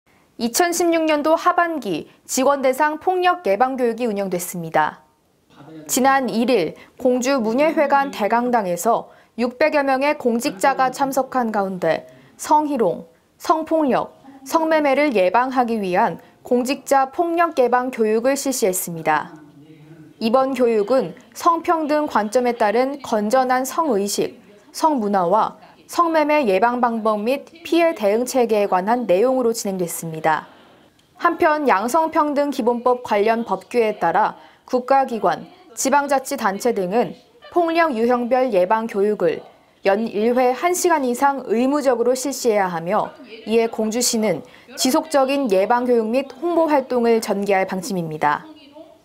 2016 하반기 직원 대상 폭력예방교육
2016년 하반기 직원 대상 폭력예방교육이 운영됐습니다.

지난 1일 공주문예회관 대강당에서 600여명의 공직자가
참석한 가운데
성희롱, 성폭력, 성매매를 예방하기 위한
공직자 폭력예방교육을 실시헀습니다.

이번 교육은 성평등 관점에 따른
건전한 성의식·성문화와 성매매 예방방법 및
피해 대응체계에 관한 내용으로 진행됐습니다.

한편 양성평등기본법 관련 법규에 따라,
국가기관, 지방자치단체 등은 폭력유형별 예방교육을
연 1회 1시간 이상 의무적으로 실시해야하며
이에 공주시는 지속적인 예방교육 및 홍보활동을
전개할 방침입니다.